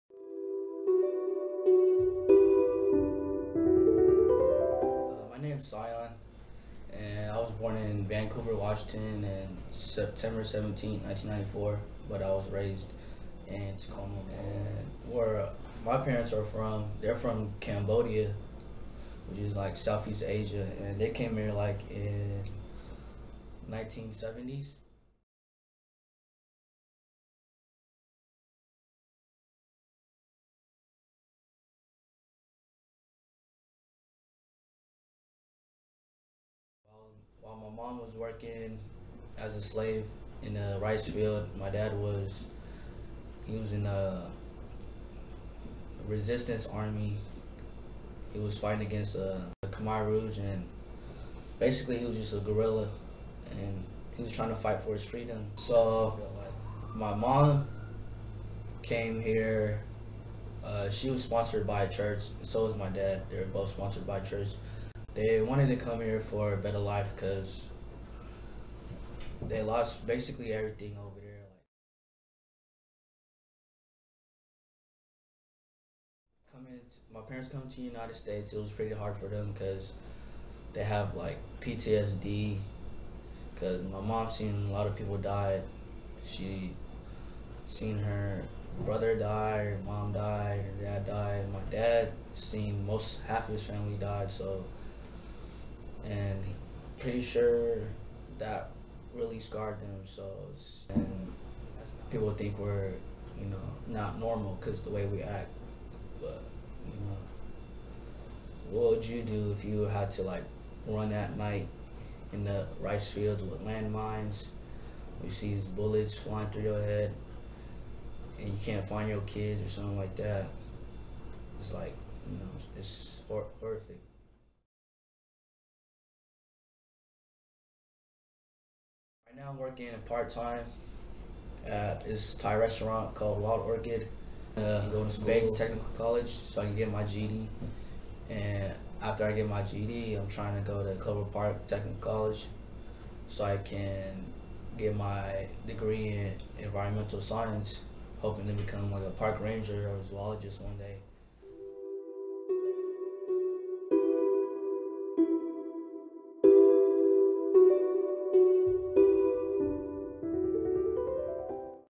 Meet Sayorn, Child of Cambodian Refugees
Sayorn is 22-years-old and the child of Cambodian refugees. His parents came here during the 1970s when the nation's ruling party at the time, the Khmer Rouge, orchestrated a genocide that killed up to 3 million people. Working through what he feels is secondary PTSD, Sayorn has a part-time job and attends Bates Technical College to get his GED. Eventually, he wants to get a degree in environmental science to become a park ranger or zoologist, open up a coconut store, and become a boxing champion of the world.